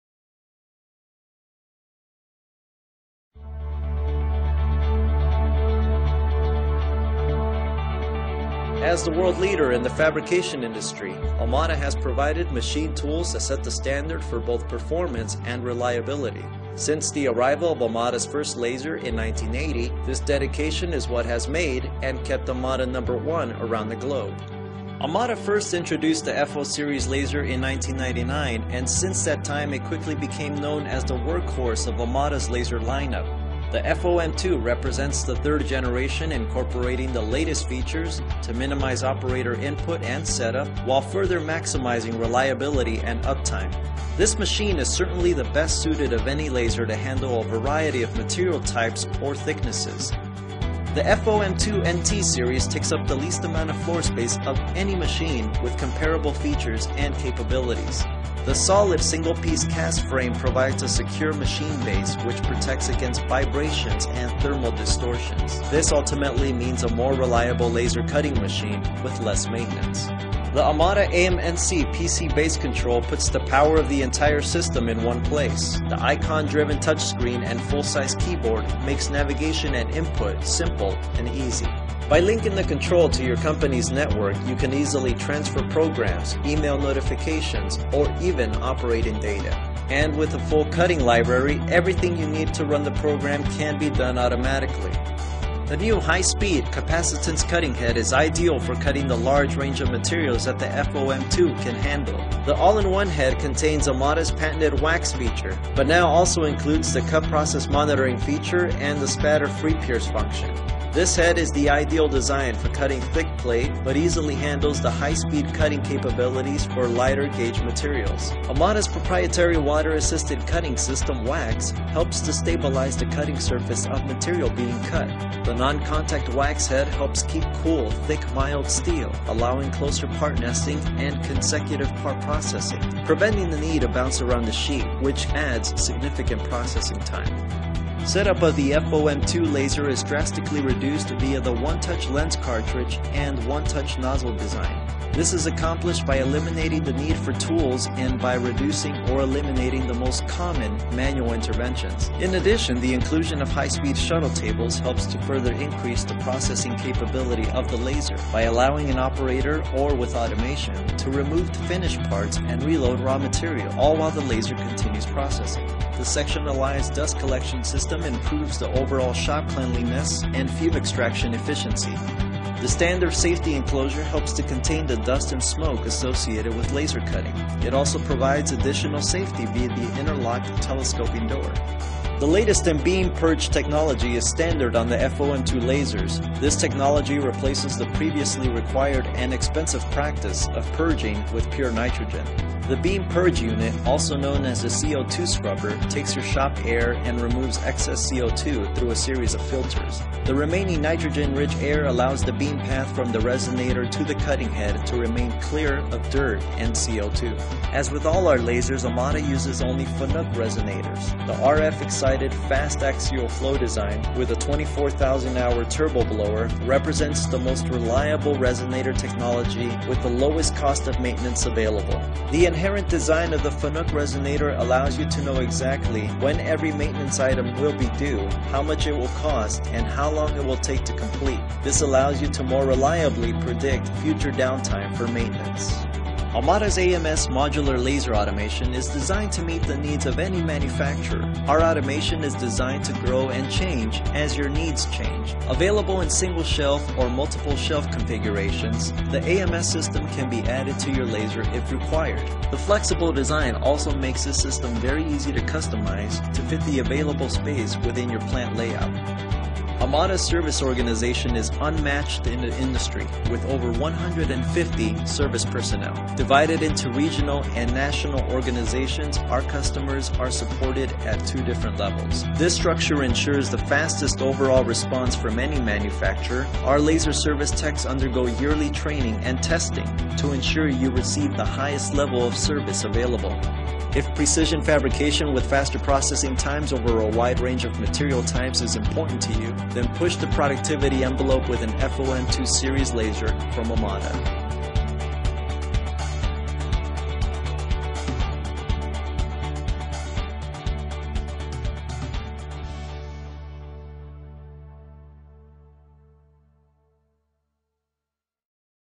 Amada LC-FOM2-laser, yleisesittely.mp4
FO MII NT-laserien mallisto edustaa Amadan hyväksi koetun, vuonna 1999 esitellyn FO-sarjan kolmatta sukupolvea. FO MII on kustannustehokkain ratkaisu laajan skaalan materiaalien yleis- leikkaukseen.